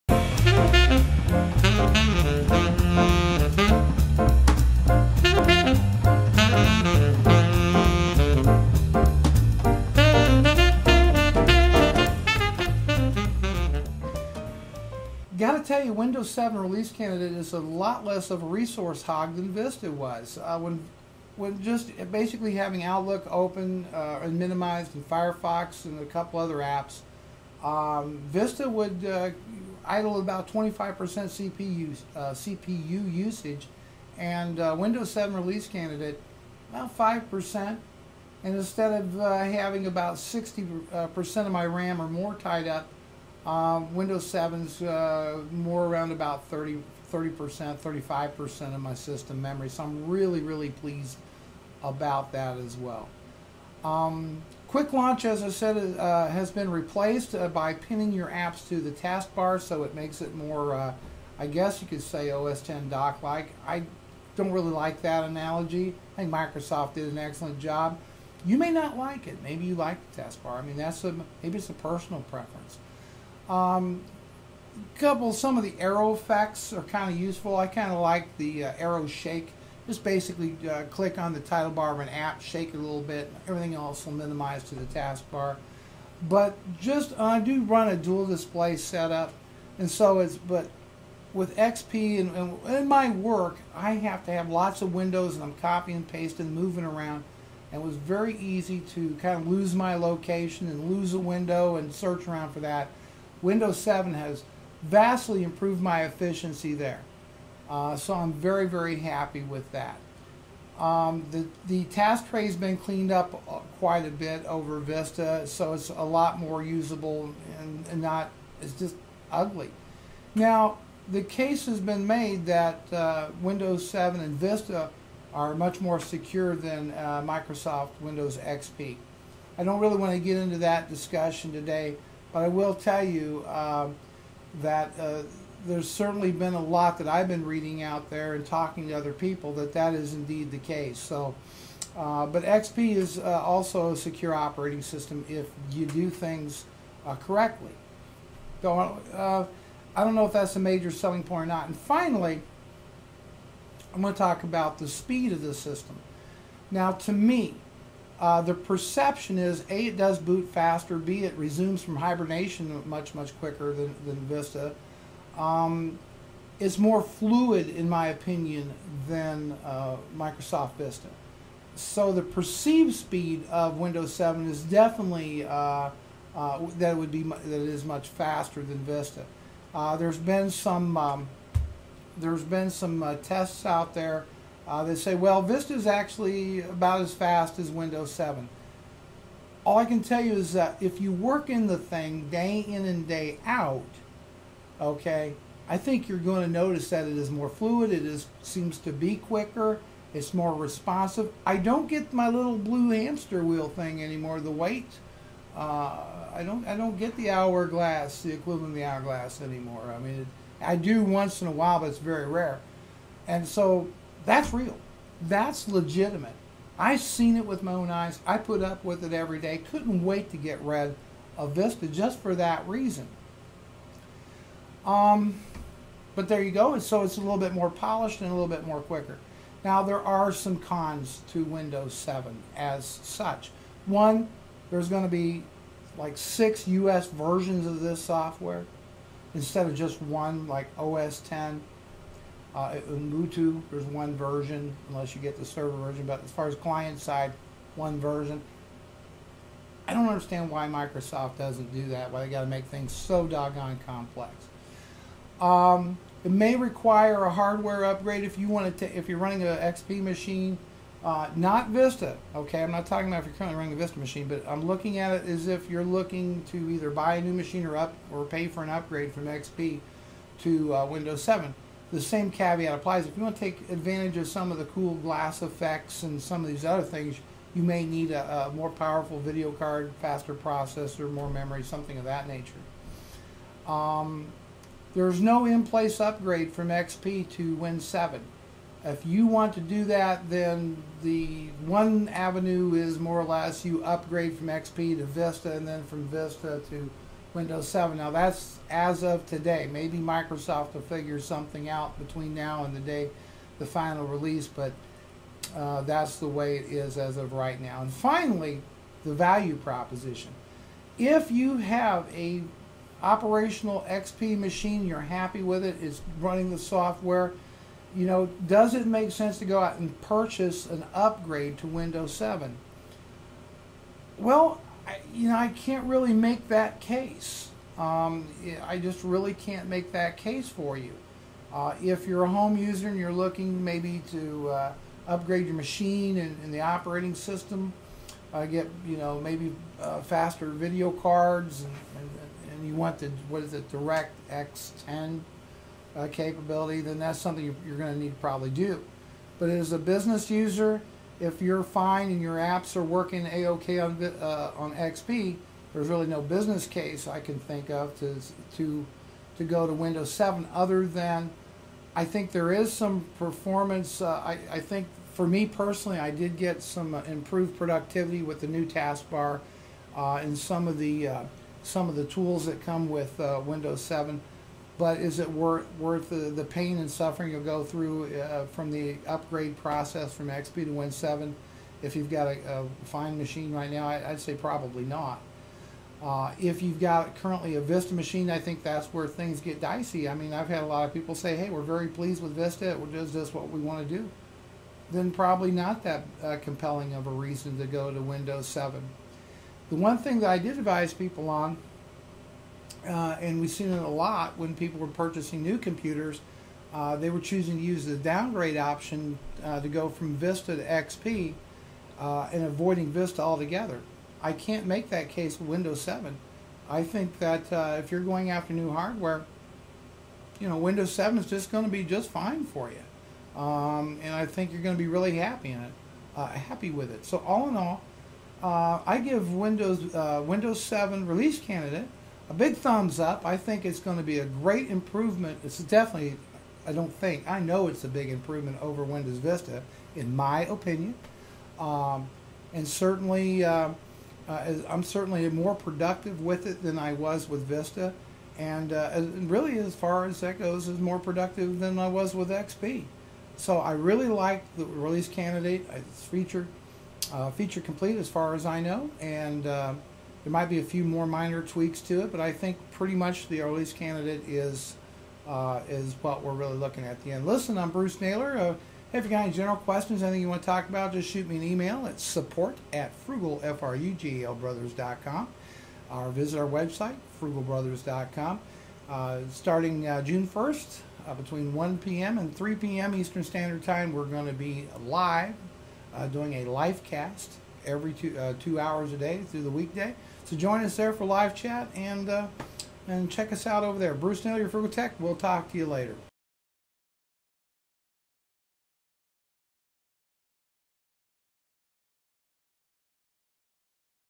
Windows 7 RC1 Review Part2- FrugalTech
Part 2 of my review of Windows RC1. I have been using this every day for my business, the last few weeks. This is what Vista should have been.  Here is link to Part2.